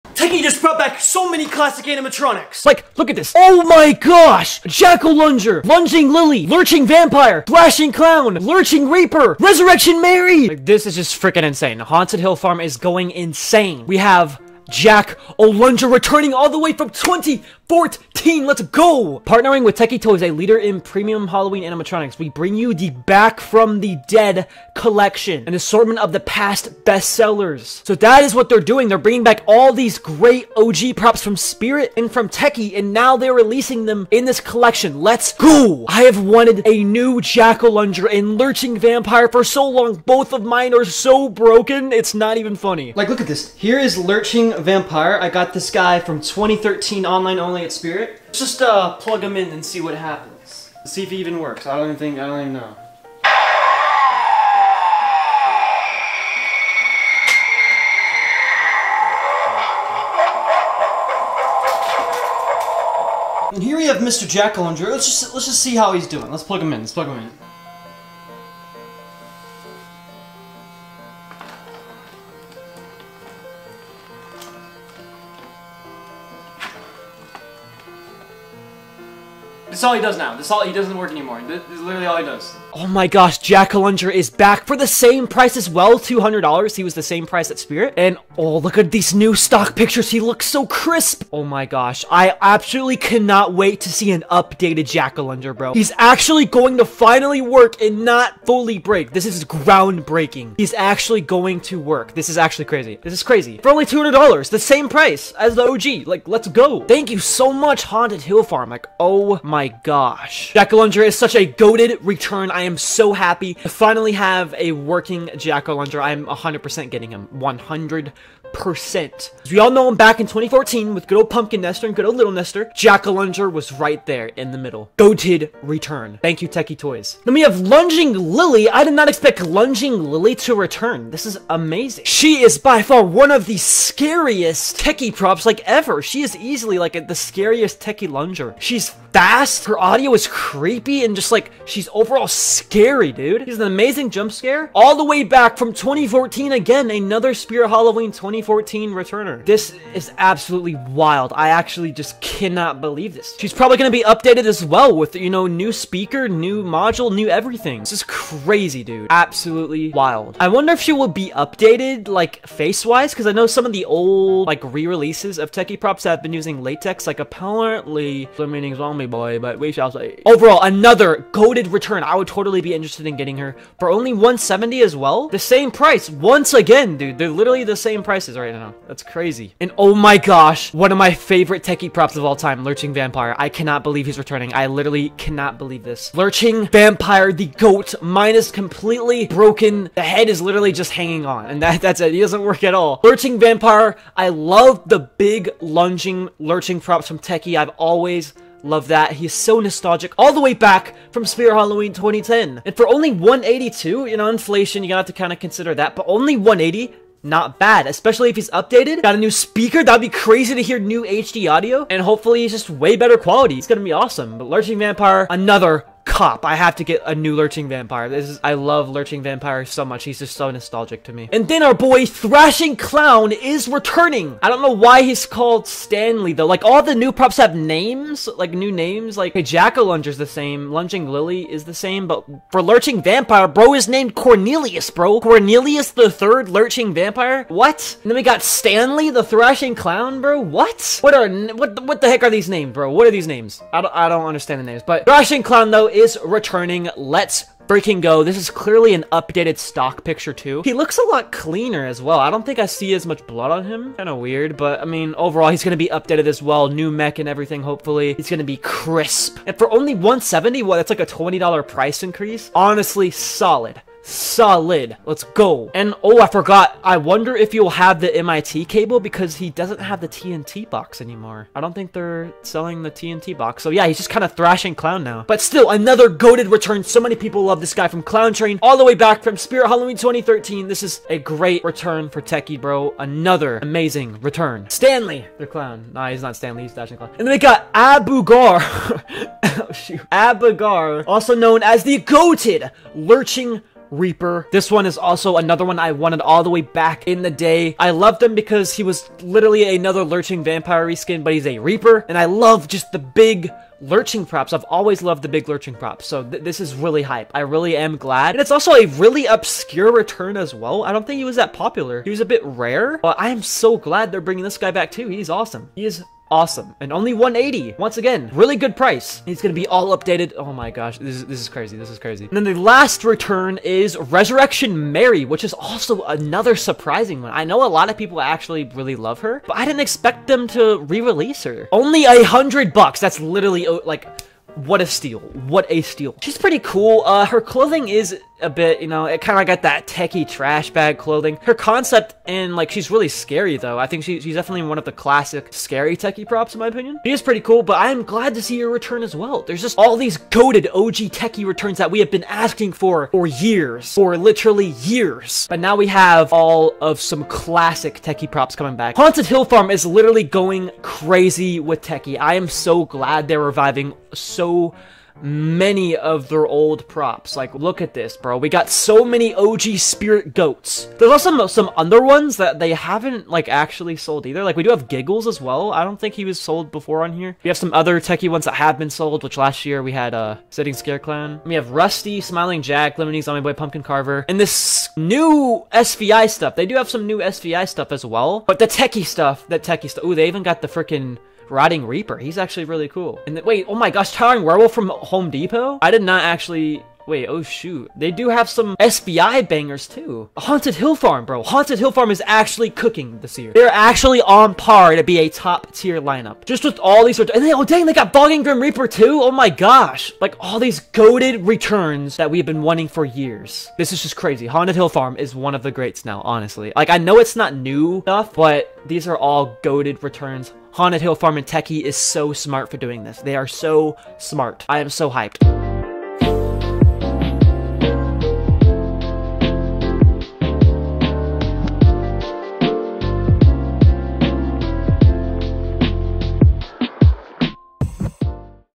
Rare Spirit Halloween Animatronics Return At Haunted Hill Farm
Rare Spirit Halloween Animatronics Lurching Vampire, Jack O lunger, Lunging Lilly, Thrashing Clown, Lurching Reaper, and Resurrection Mary all return at Haunted Hill Farm for Halloween 2023!